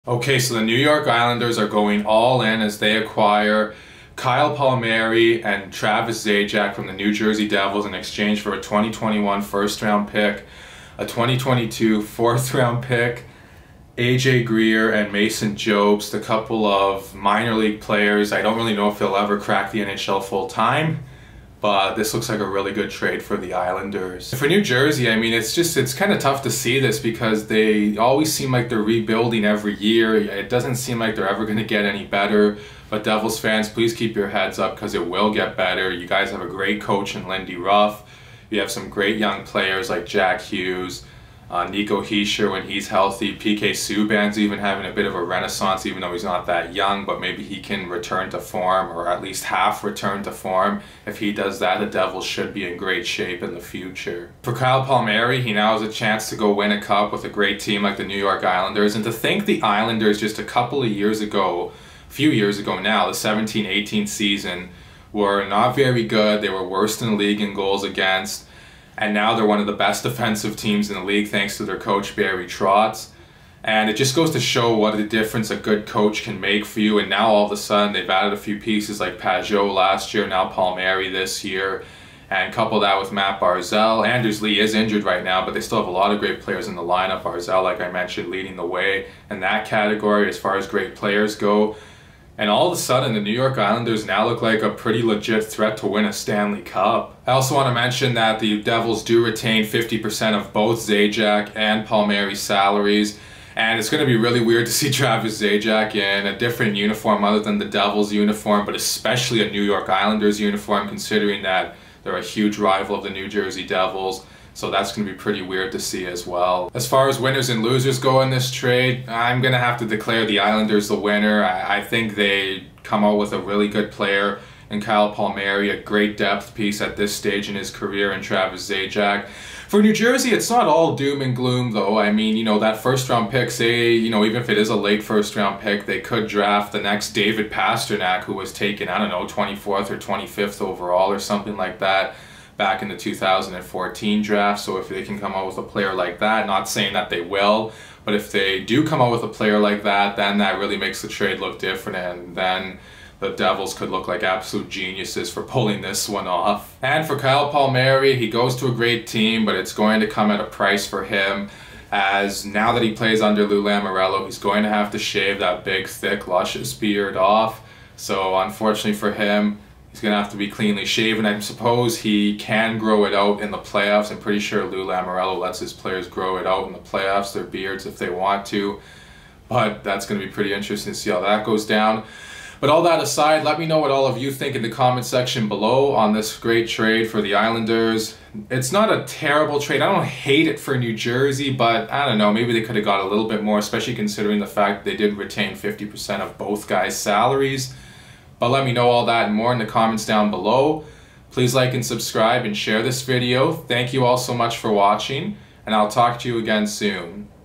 New York Islanders acquire Kyle Palmieri and Zajac from New jersey for draft picks and prospects
NewyorkIslanders NewjerseyDevils KylePalmieri TravisZajac NHL The Islanders look pretty dnagerous heading into the playoffs with Palmieri... Who do you think won the trade?